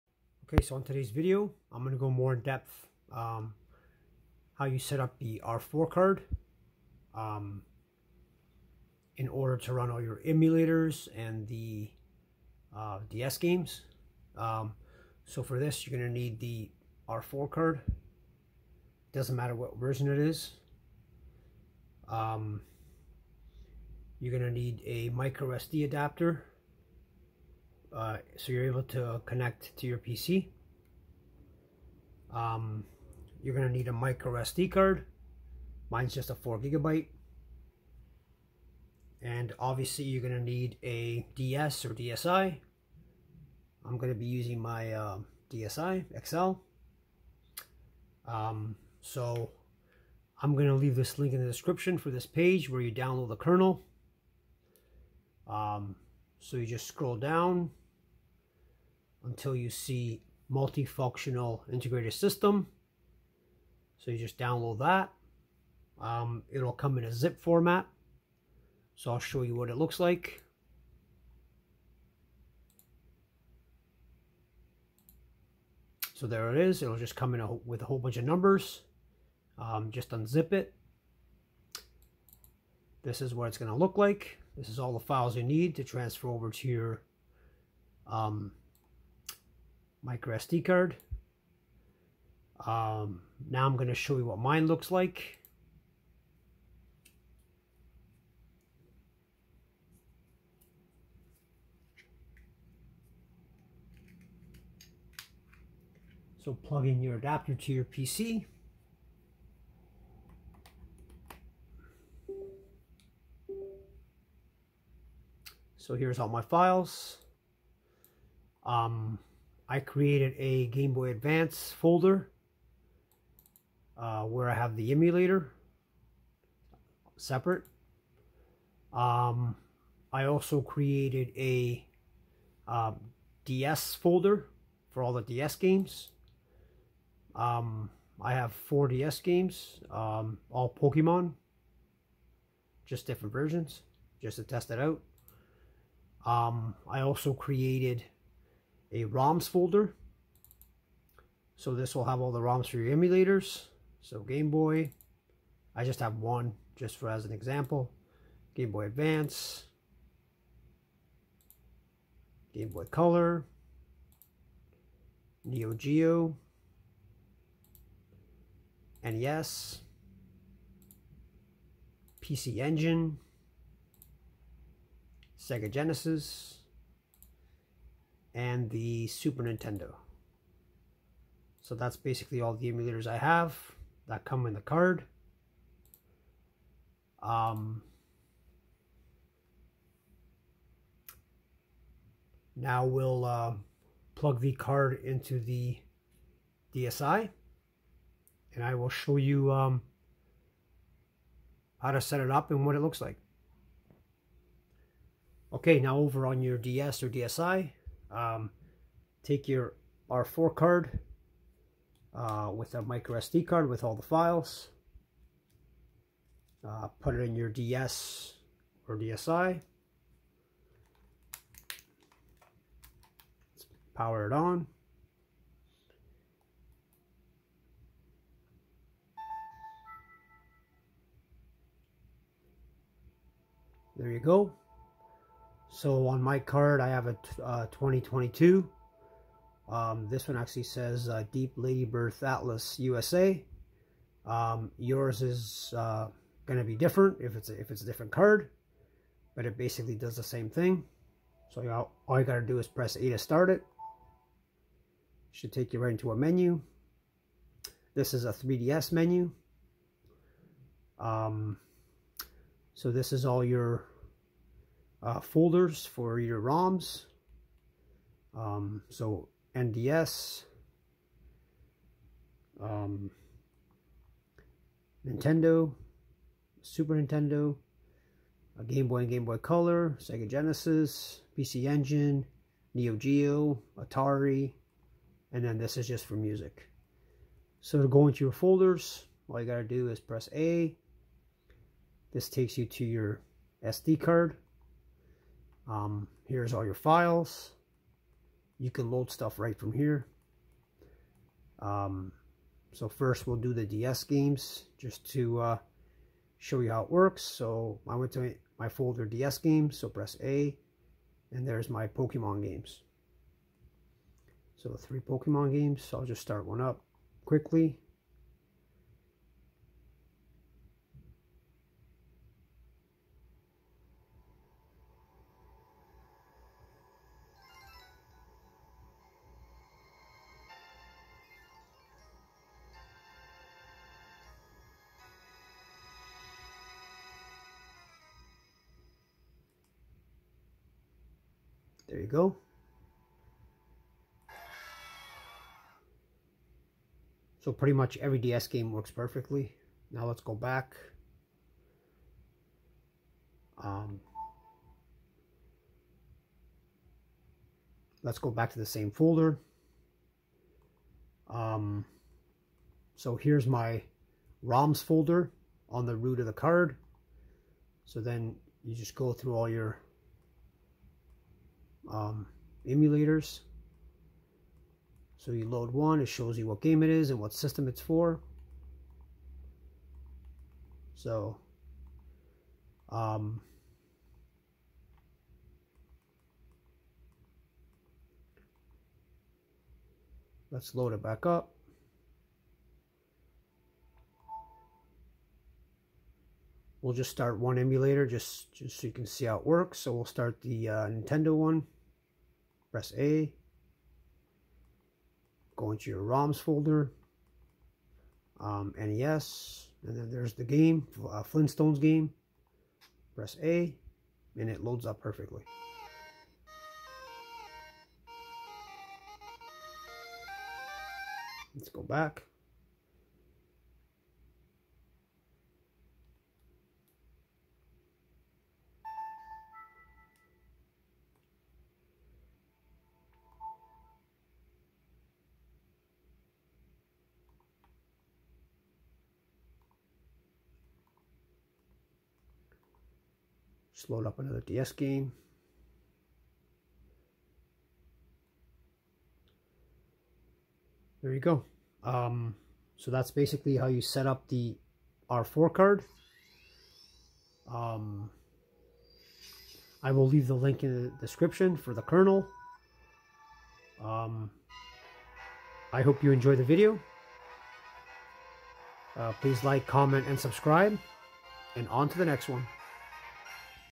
How to setup the R4 Card to run Emulators & Play Ds Games on Nintendo Ds / 2Ds or 3Ds!
Setting up the R4 Card to run emulators & play Ds Games on Nintendo Ds / 2Ds or 3Ds Systems!